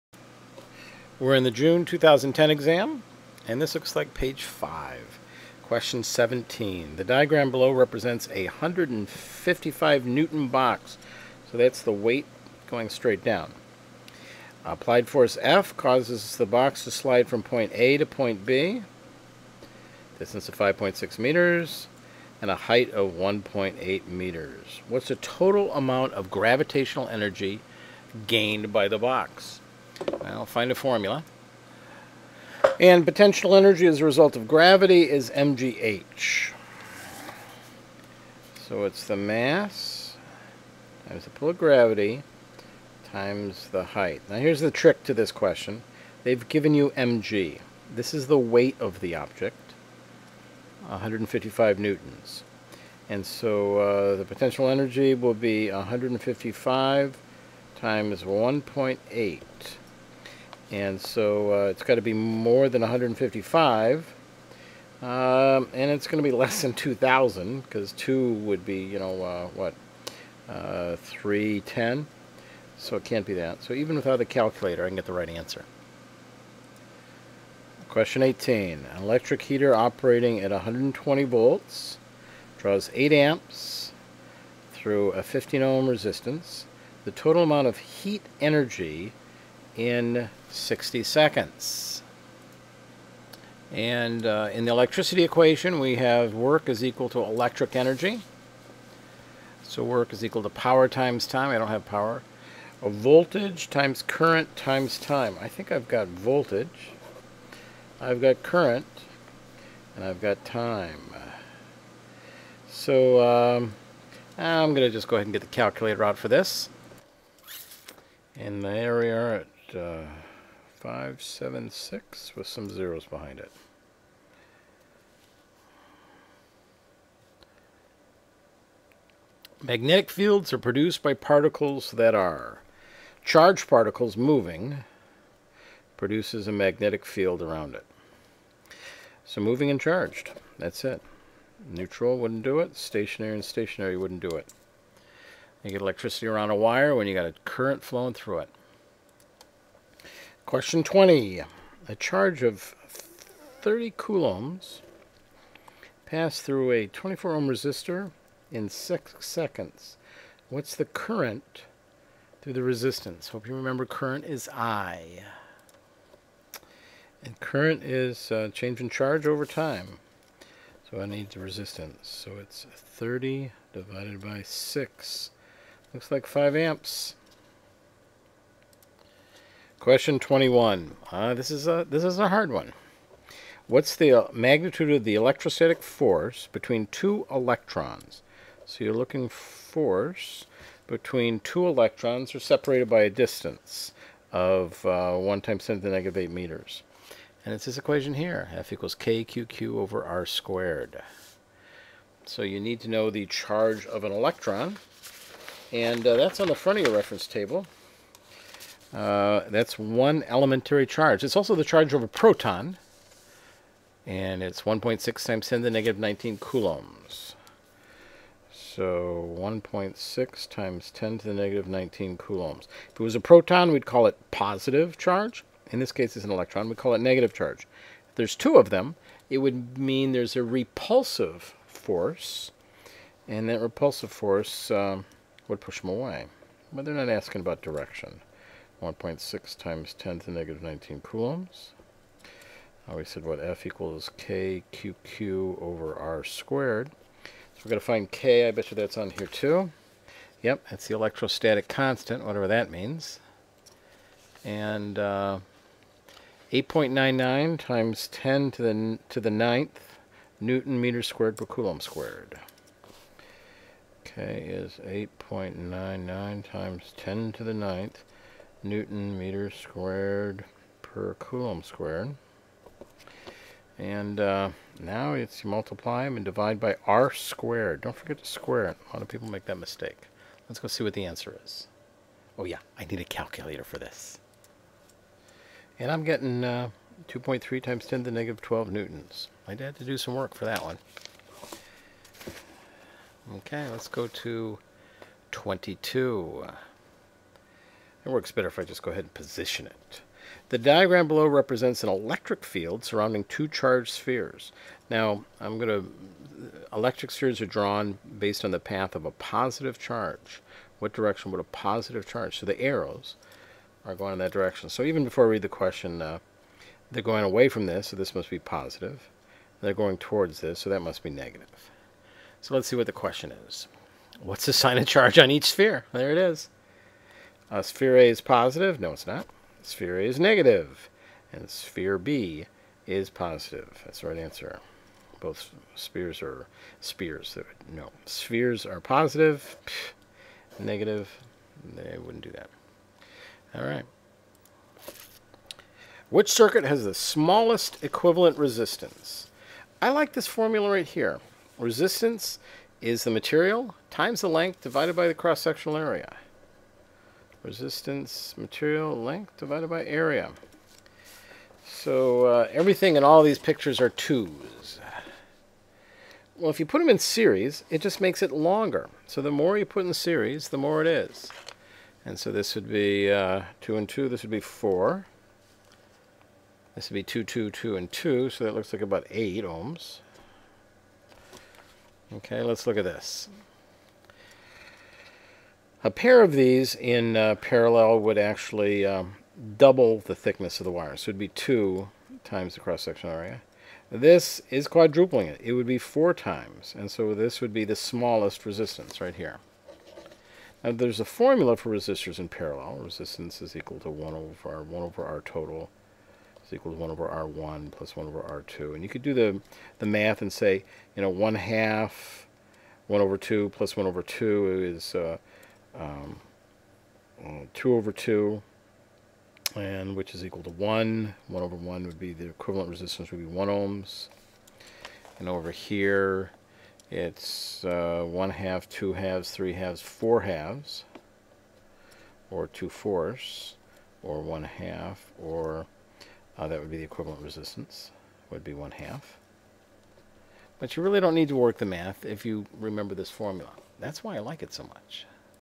NYS Regents Physics: June 2010, Pg 5
June 2010. Page 5. Questions 17-23
to download a pdf file. Print out a copy and follow along. (when it is posted)